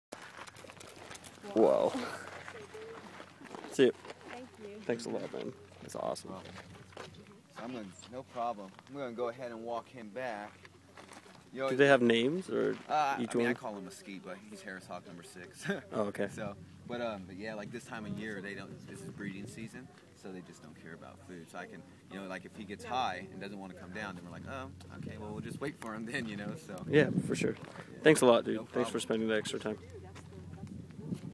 Harris hawk show at The Desert Museum
The friendly, knowledgeable staff at the Desert museum explain and display the hunting methods and social structure of Harris Hawks!